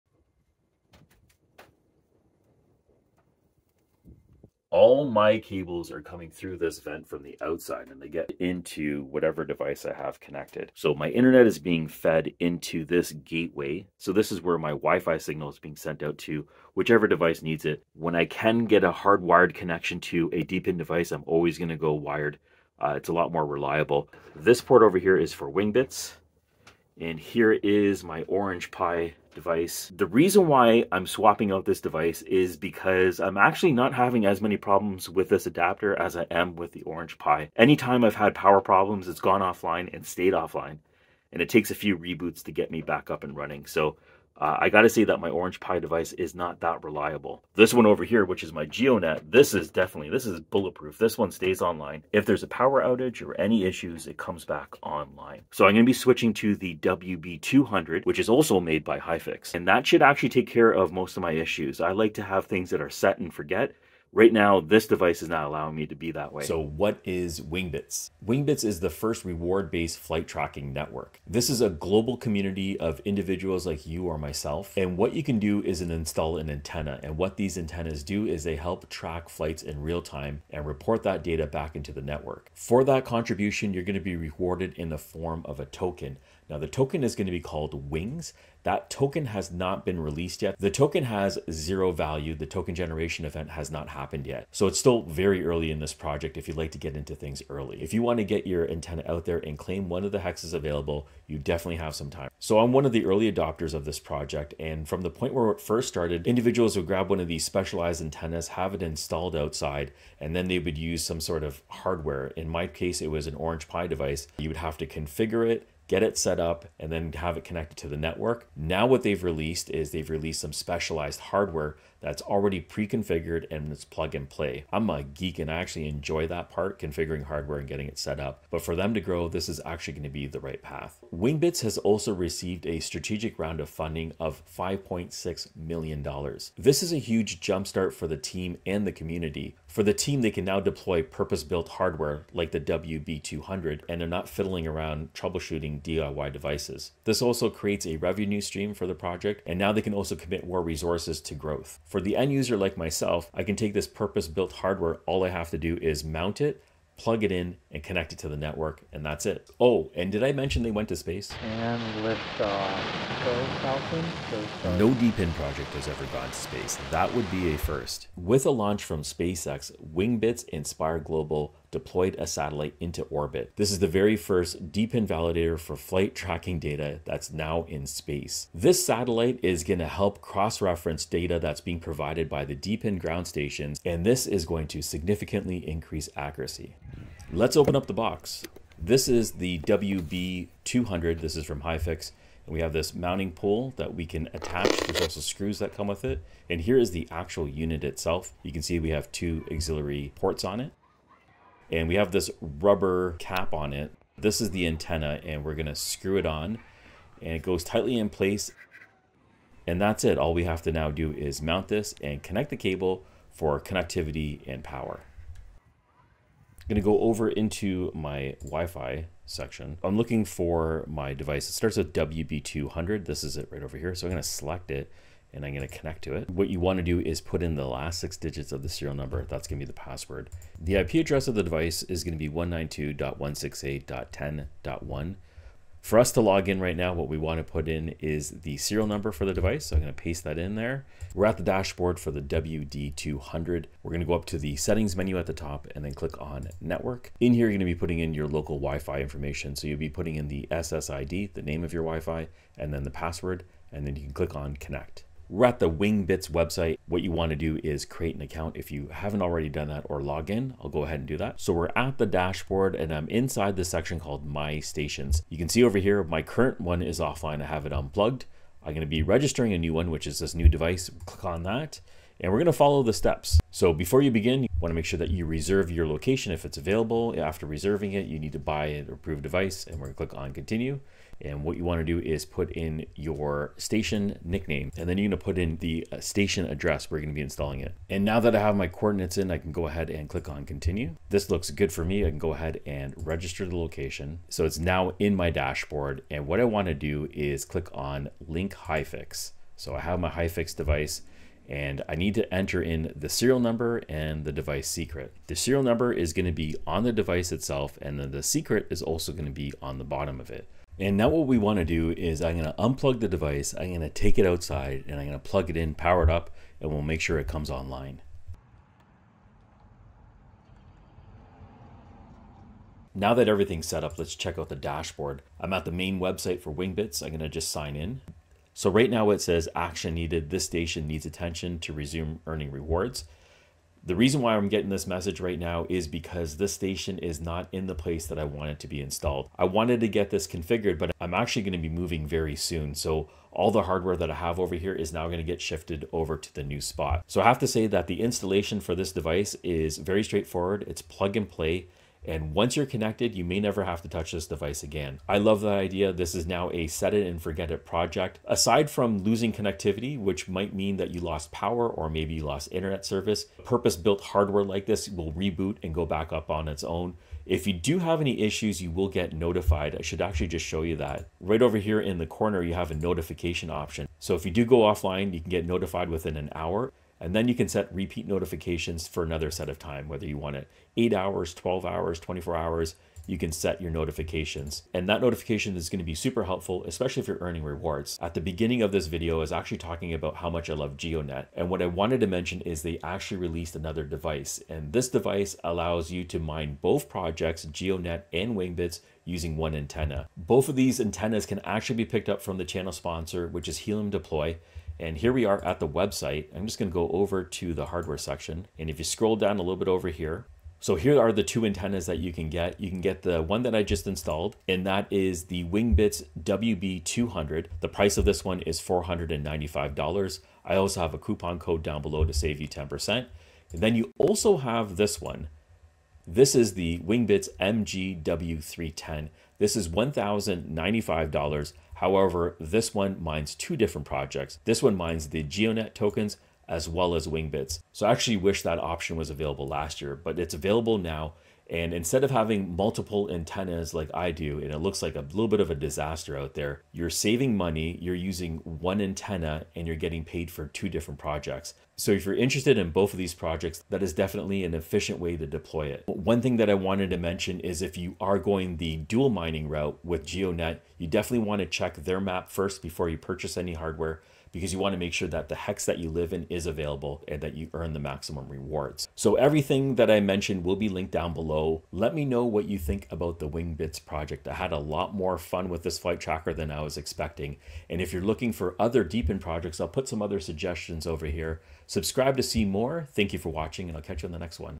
Wingbits Setup Just Got an Upgrade – Here’s What Changed 🚀
In this video, I delve into the Wingbits WB200, a plug-and-play ADS-B flight tracking station that allows you to earn $WINGS tokens by contributing real-time aviation data to the decentralized Wingbits network. I guide you through the unboxing, installation, and setup process, sharing my personal experiences and insights. We also discuss the advantages and disadvantages of the WB200 compared to DIY setups, and explore its potential impact on global flight tracking and aviation safety. Whether you're an aviation enthusiast or looking for a new way to earn crypto, this video provides a comprehensive overview of the Wingbits WB200

🔗 Step by Step Setup Guide